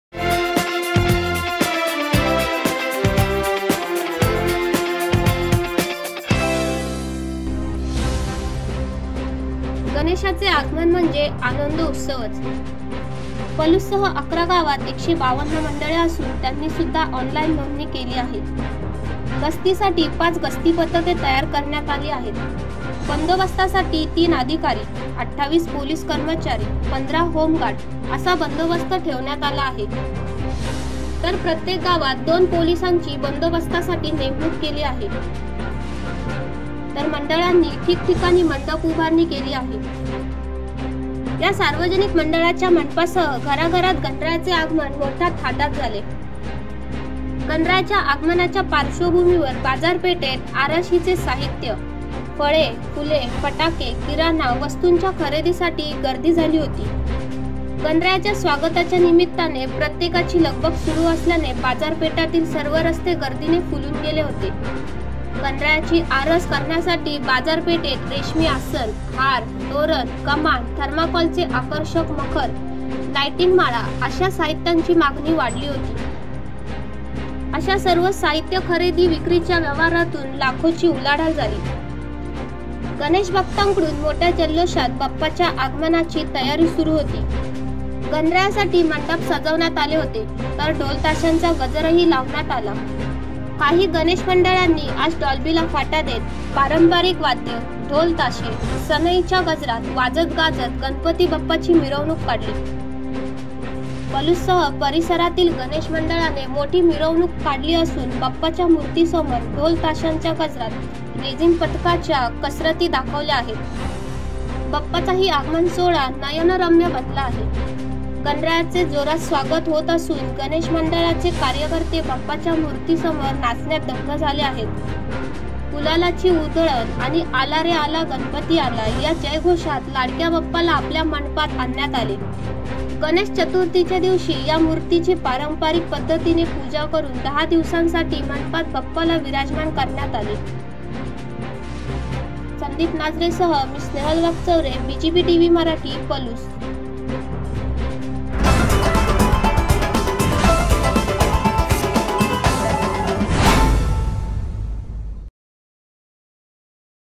पलुस तालुक्यातून पाच दिवसांच्या बाप्पांना निरोप.
BGB Tv Marathi | Special Report | 19 Sep 2018 |

Reporter - Amar Mulla

Voice Over - Snehal Wakchoure

Editor - Snehal Wakchoure